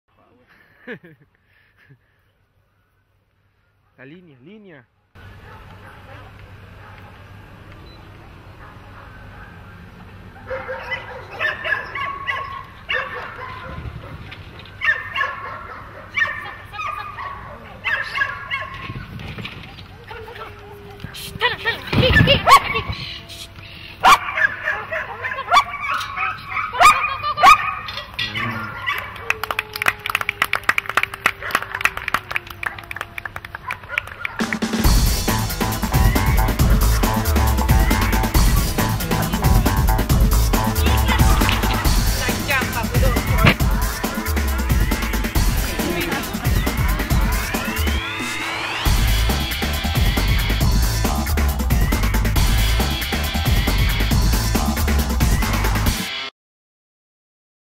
Jenny Damm en Chile - Club Agility Chilcoa Octubre del 2010
Jenny Damm en una pasada con la Border Collie Amelia de Chile... Además promocion a las 2 horas de los mejor del seminario realizado en el Club Agility Chilcoa en octubre del 2010.